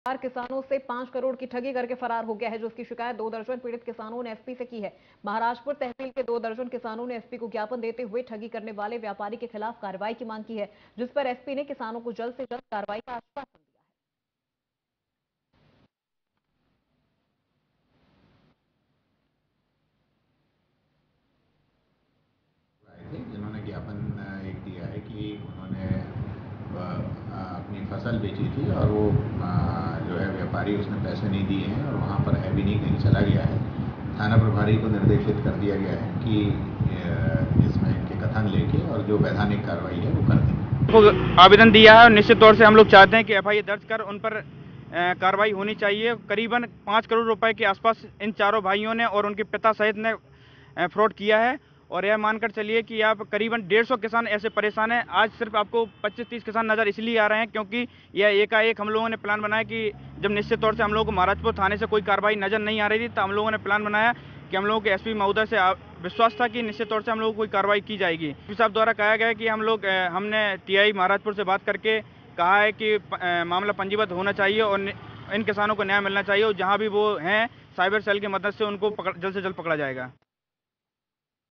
Chhatarpur News : व्यापारी परिवार ने दो दर्जन किसानों को बनाया शिकार | Hindi News
Chhatarpur  News : किसानों से ठगी का मामला ,व्यापारी परिवार ने दो दर्जन किसानों को बनाया शिकार | Hindi News